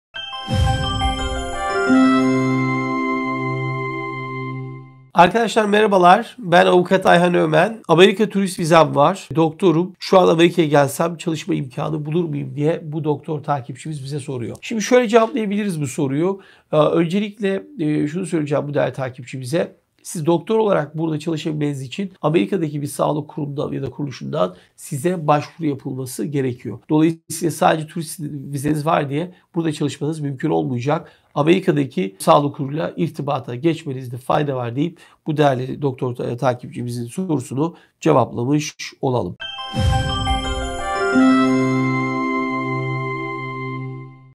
TURİST VİZESİ SAHİBİ DOKTORLAR AMERİKA’DA NASIL ÇALIŞABİLİR ?
Soru: Amerika turist vizem var, doktorum. Şu an Amerika’ya gelsem çalışma imkânı bulur muyum ?